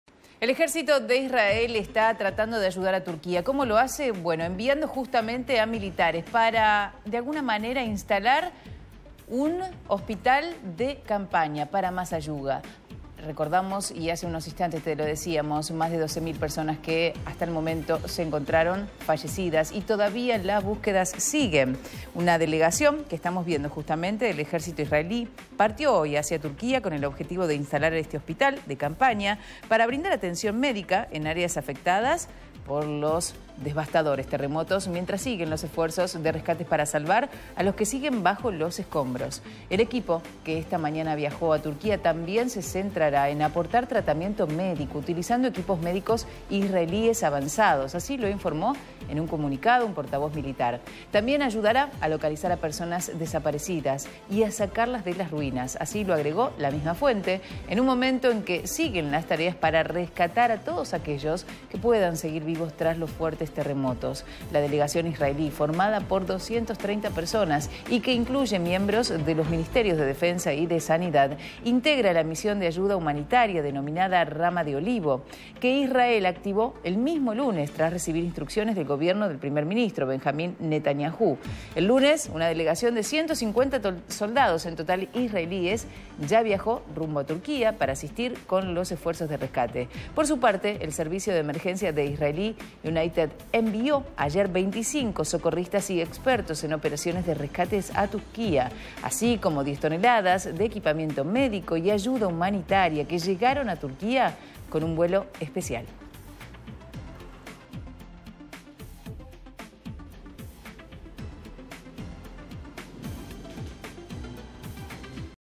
El Ejército de Israel viajó a Turquía para instalar un hospital de campaña
👉🏻 Una delegación del Ejército israelí partió hacia Turquía con el objetivo de instalar un hospital de campaña para brindar atención médica en áreas afectadas por los devastadores terremotos.